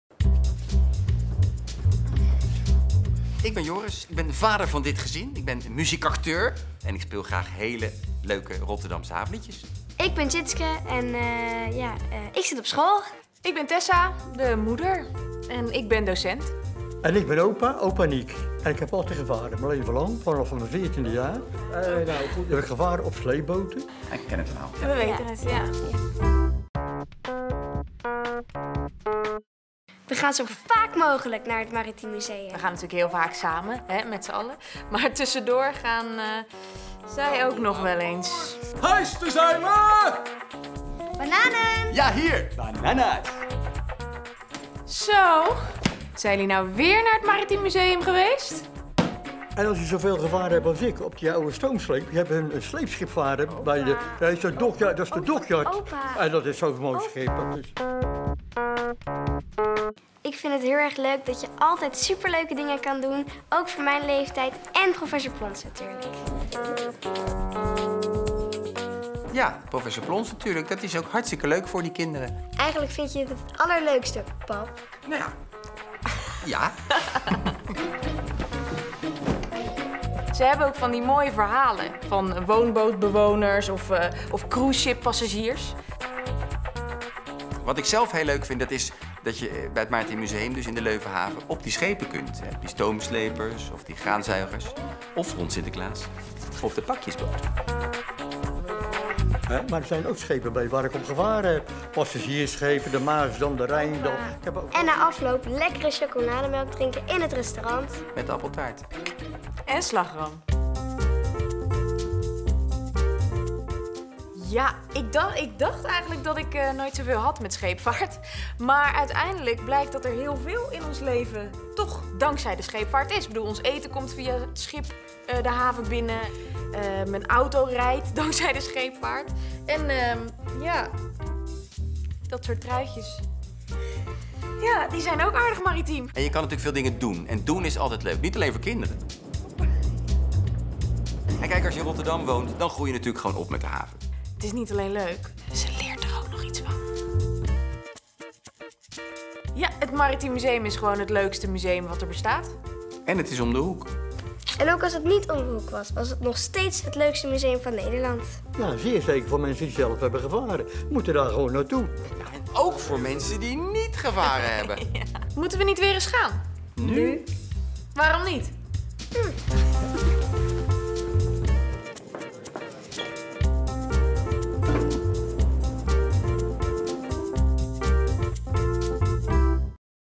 De familie vertelt: wat is er zo leuk aan het Maritiem Museum?
De familie vertelt ... iedereen heeft wel een reden om naar het Maritiem Museum te gaan. Historische topstukken, Professor Plons, smeuige verhalen, de heerlijke chocomel en de vele activiteiten die je  binnen en buiten kan doen in de haven!